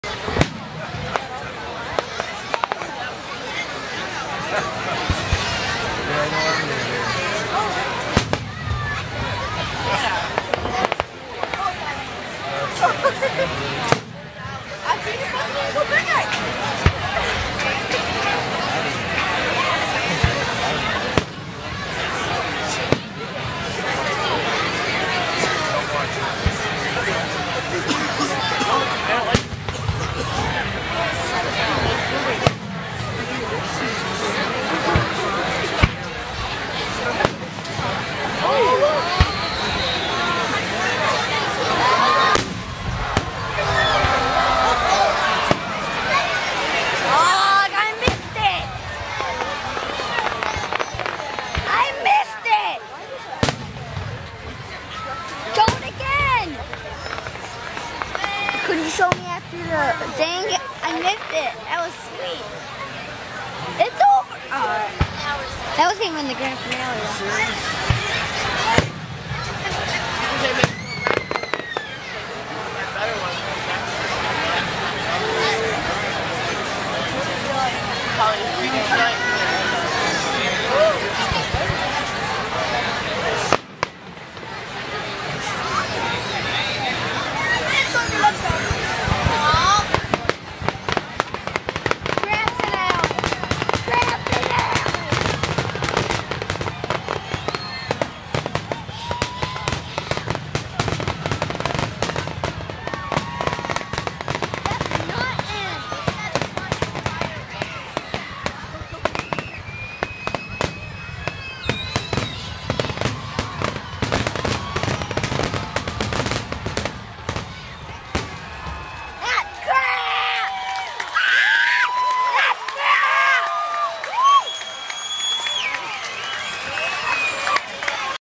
Wyandotte Fireworks 2011 Grand Finale
Except for the child commentary at the end, this turned out to be a great video clip. enjoy!